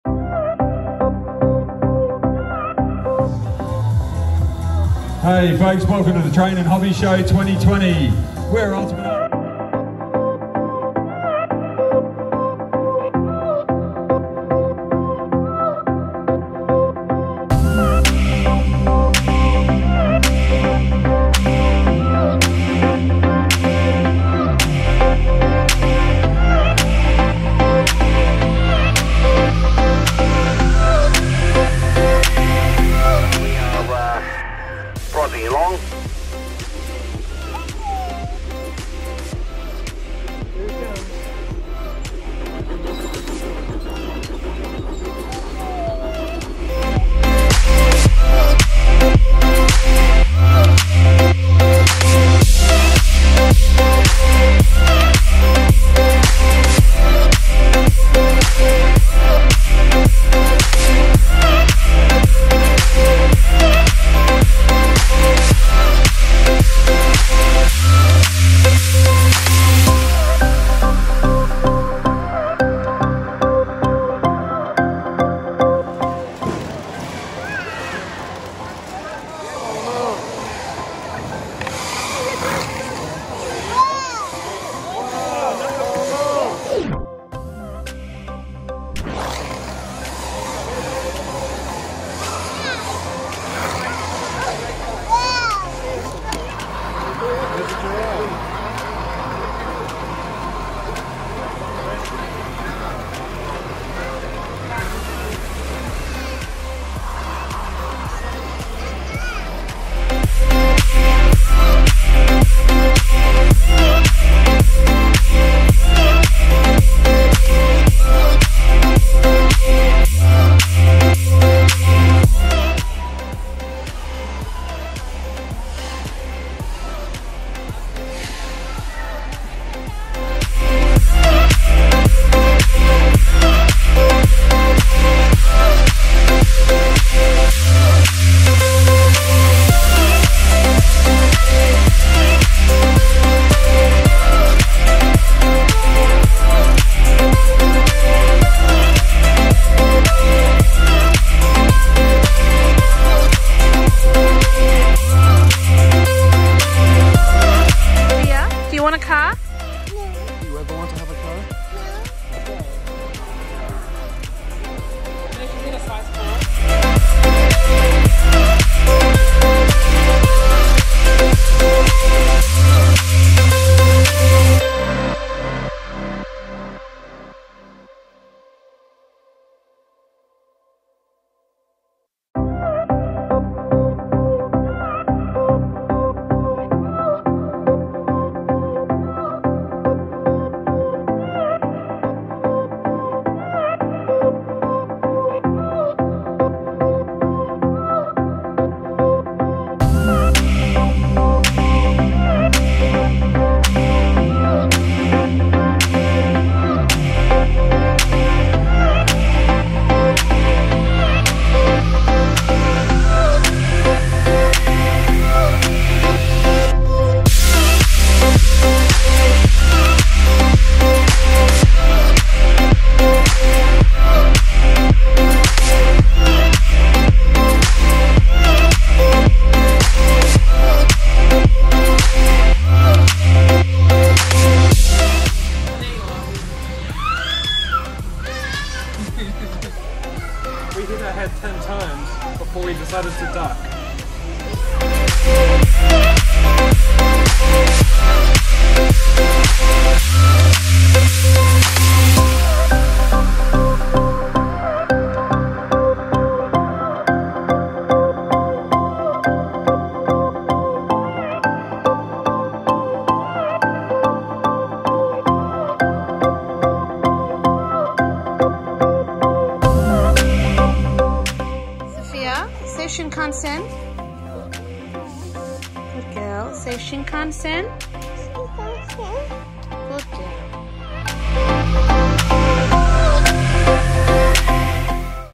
Melbourne Train & Hobby Show, LEGO Shopping, Baby's Shinkansen Moment & Aquarium Fun! 🚂🧱🐟
Hello, Melbourne Explorers! In today's adventure-packed video, we take you along as we visit the fascinating Melbourne Train & Hobby Show, indulge in some irresistible LEGO shopping, celebrate our baby's adorable "Shinkansen" moment, and end the day with an underwater experience at the Melbourne Aquarium. Get ready for a day full of excitement and discoveries! 🚂🧱🐟

Our journey begins at the Melbourne Train & Hobby Show, where we explore the incredible displays of model trains, hobby crafts, and more. Join us as we marvel at the creativity and intricate details of this fantastic event! 🚂🛤️

In an unforgettable moment, our baby finally says "Shinkansen," melting our hearts and filling us with joy.

Later, we dive into the mesmerizing world of the Melbourne Aquarium, where we encounter fascinating marine life and learn about the importance of ocean conservation. Swim along with us through the vibrant underwater world and share this incredible experience! 🐟🐠🦈